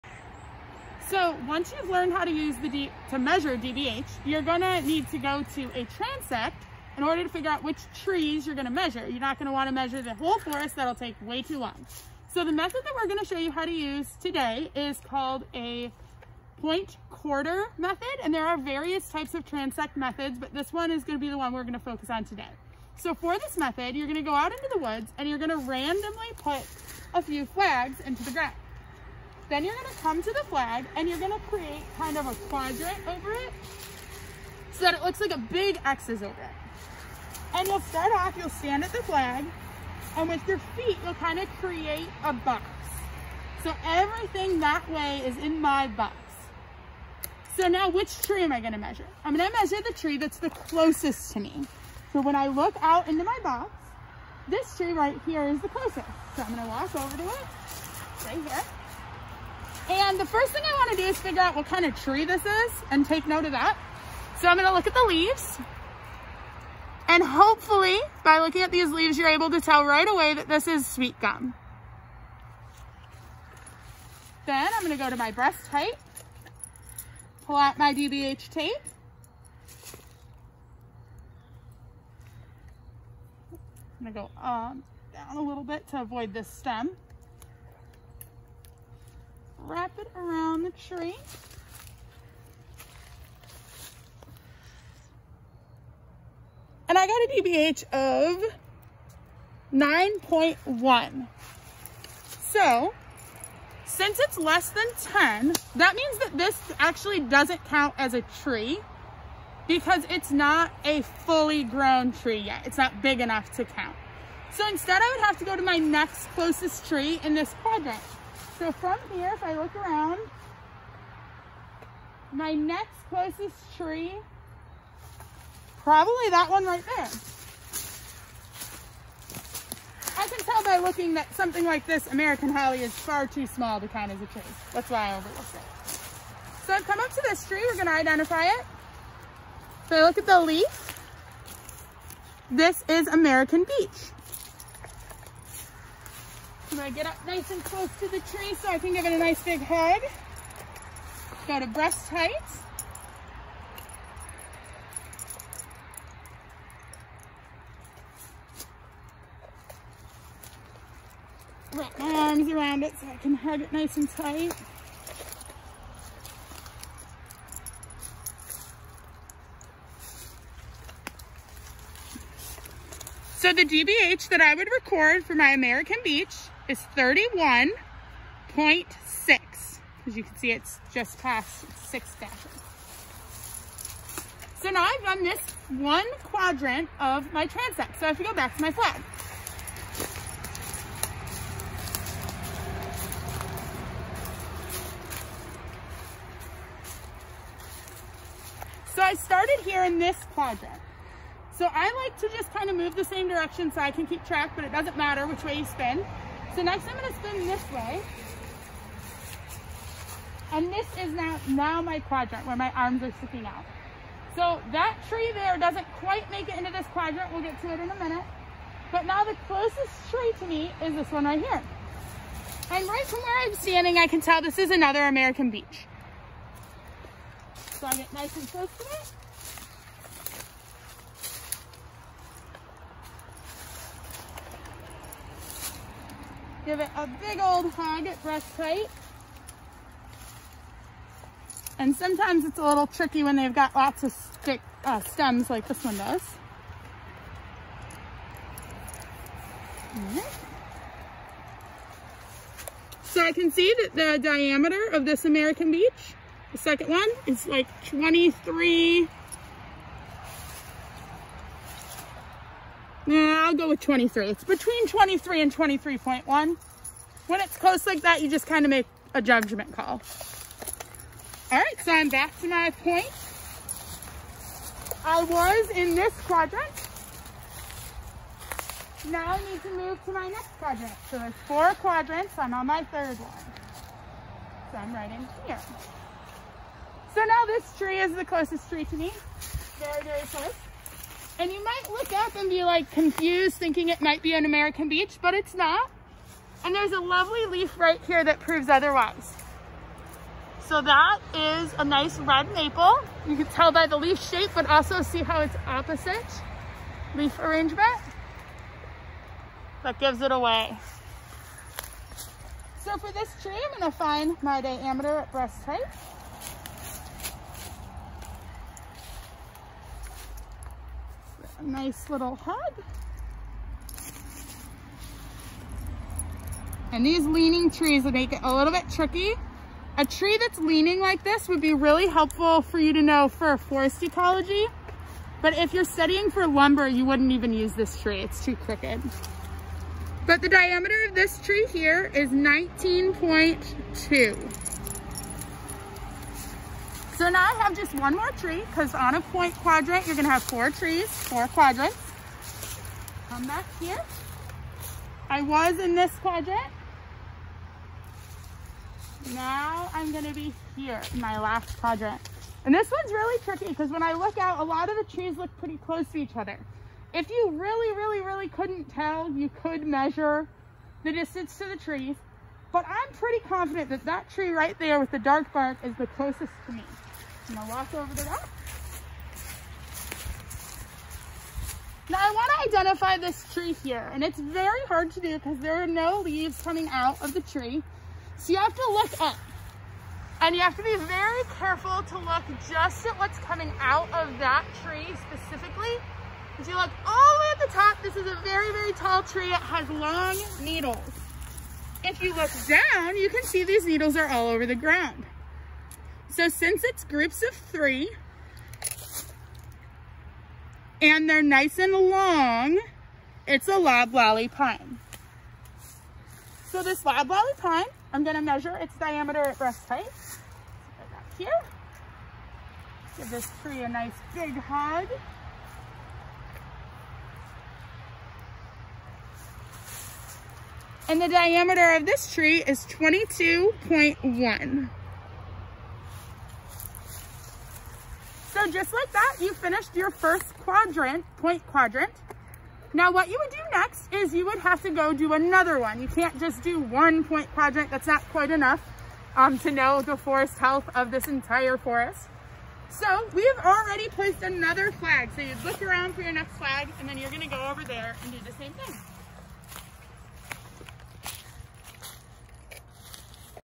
Point-Quarter Transect Method
This video goes through how to gather data at one point of a transect using the Point-Quarter Method.